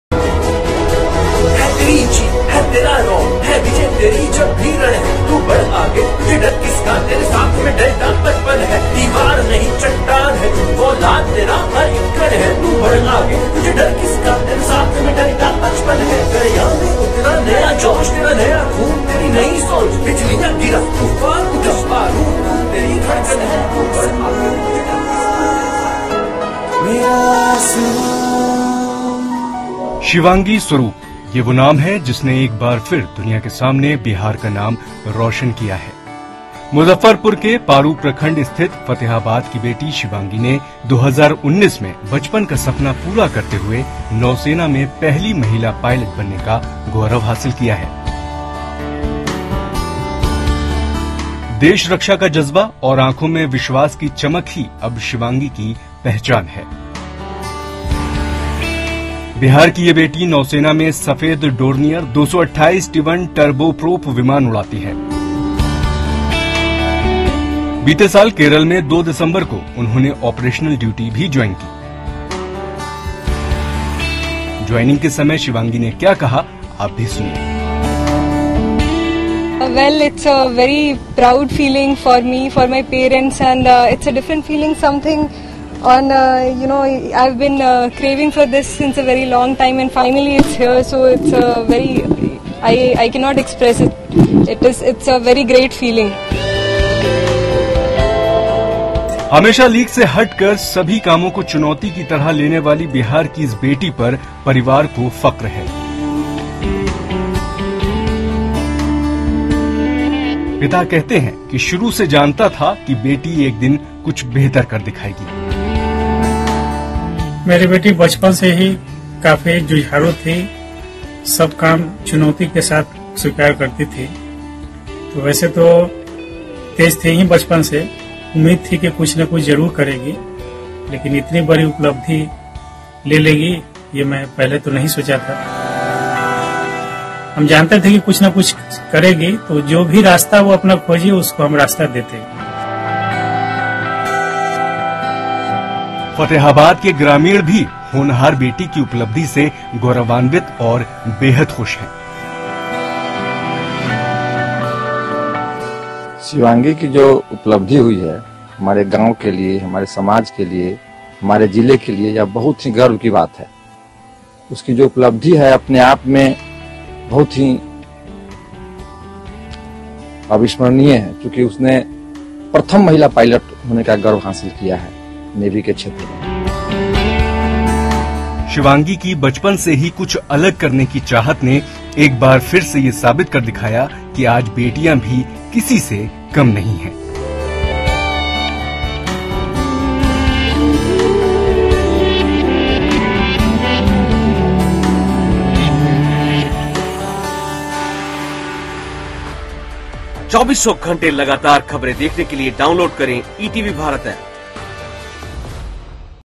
शिवांगी स्वरुप, भारतीय नौसेना की पहली महिला पायलट | Shivangi Swaroop, first Naval Woman Pilot
शिवांगी स्वरुप...ये वो नाम है, जिसने एक बार फिर दुनिया के सामने बिहार का नाम रोशन किया। बिहार की इस बेटी ने 2019 में बचपन का सपना पूरा करते हुए, नौसेना में पहली महिला पायलट बनने का गौरव हासिल किया !